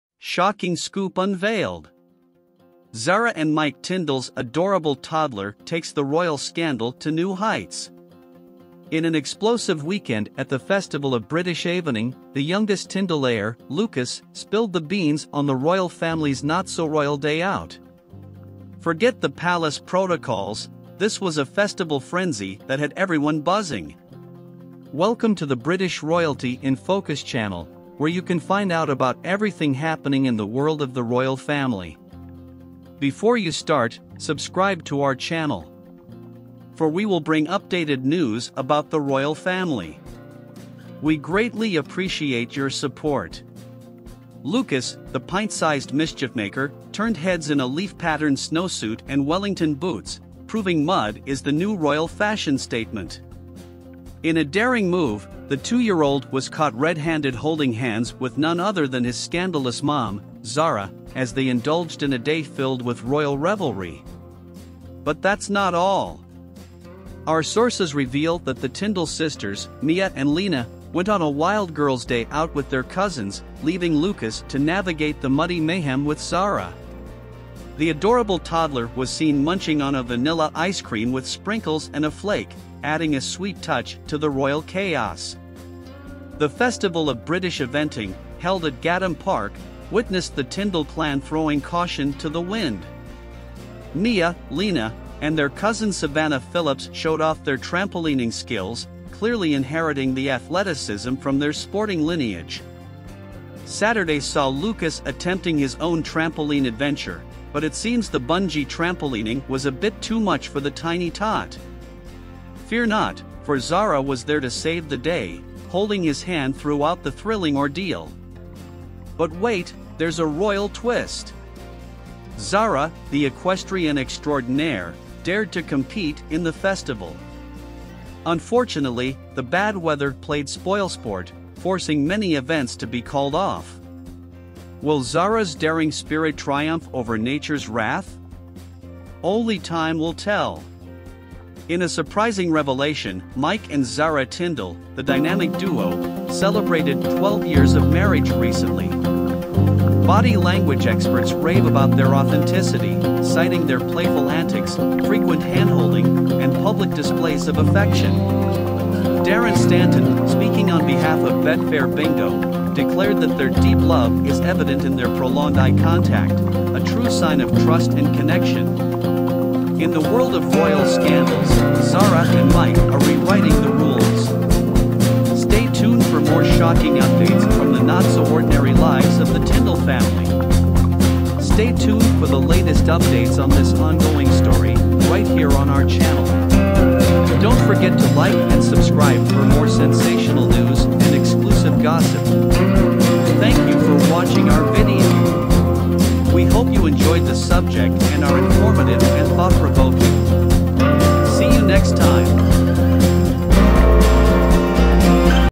UNVEILED! Zara and Mike Tindle's Adorable Toddler Takes the Royal Scandal to New Heights
Get ready for a royal rollercoaster as we spill the deets on Zara and Mike Tindle's youngest, Lucas, turning a casual day out into a scandalous royal affair! From mud-splattered fashion to sweet family moments, we've got the inside scoop.
Don't miss this royal escapade filled with rebellion, scandal, and unexpected moments from the not-so-ordinary lives of the Tindle family. It's a video fit for royalty – click play now! 👑🎥

00:00 - UNVEILED! Zara and Mike Tindle's Adorable Toddler Takes the Royal Scandal to New Heights
00:47 - Subscribe to the channel
02:30 - Leave your like